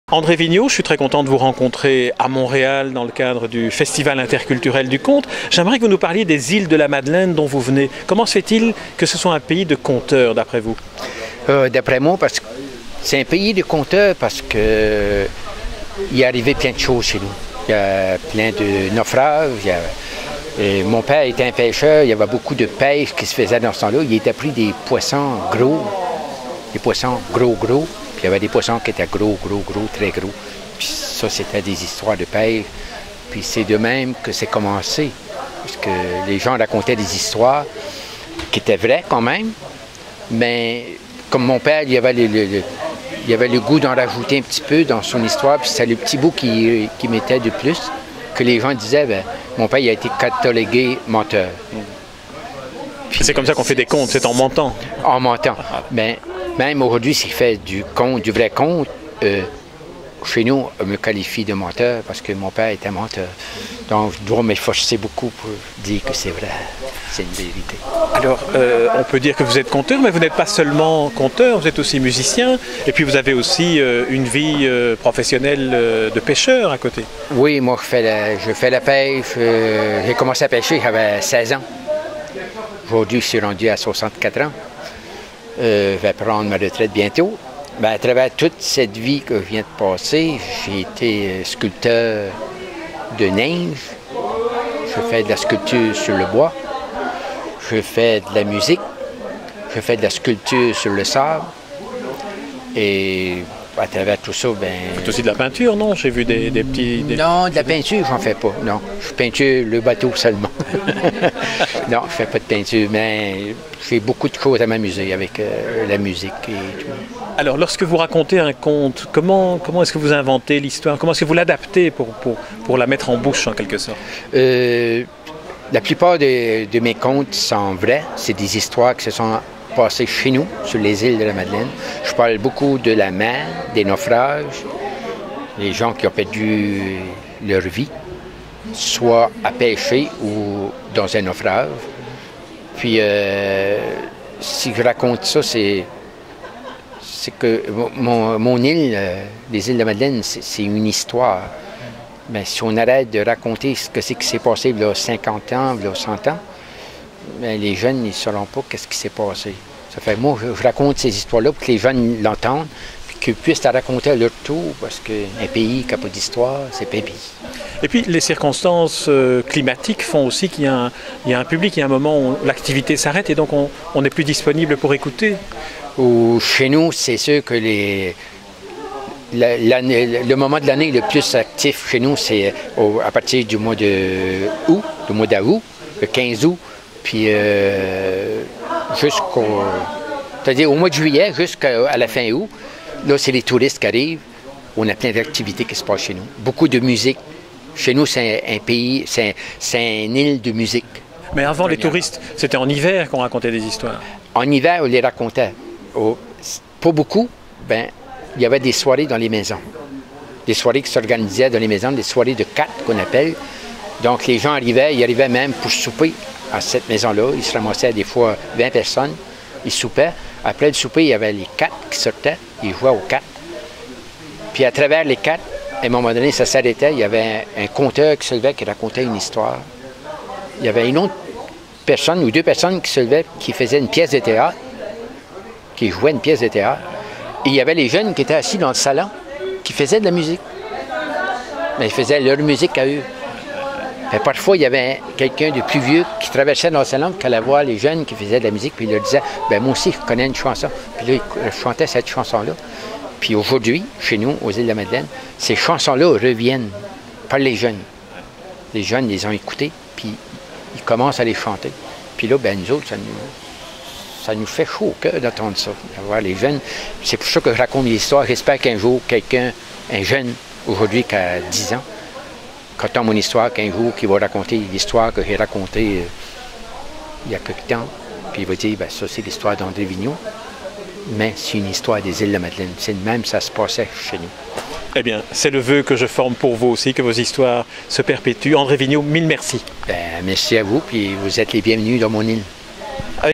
André Vigneau rencontré au Festival interculturel du conte du Québec
Entrevue avec André Vigneau, conteur et artiste multidisciplinaire des Îles-de-la-Madeleine, rencontré le 20 octobre 2013 dans le cadre de la 12e édition du Festival interculturel du conte du Québec, qui se déroule du 18 au 27 octobre 2013.

Entrevue réalisée par Edmond Morrel pour le FICQ
Caméra : Jean Olivier